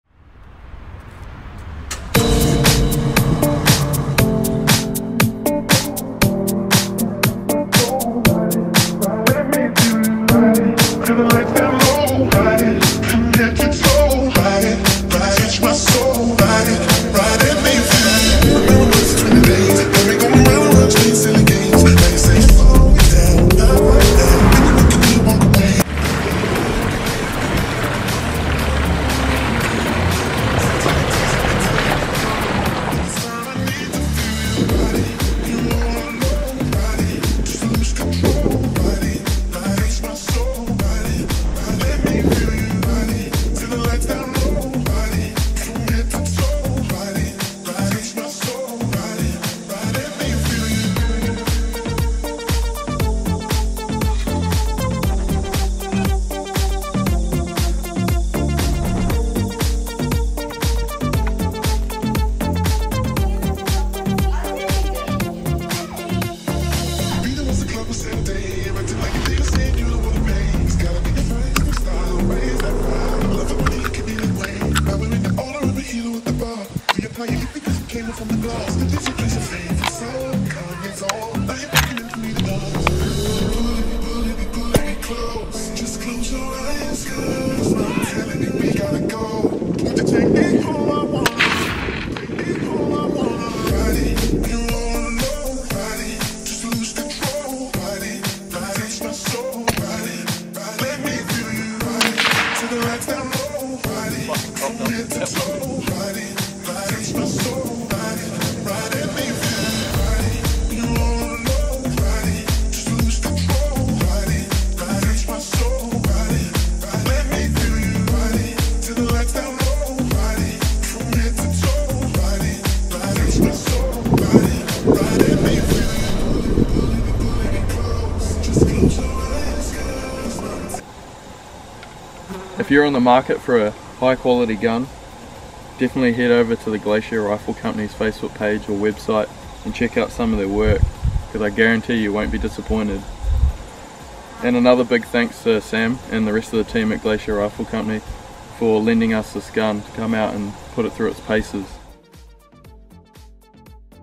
The best hunting rifle in NZ (GRC) .308 Bush Hunter
the glacier rifle company (GRC).308 Bush Hunter weighing in at only 2.3kg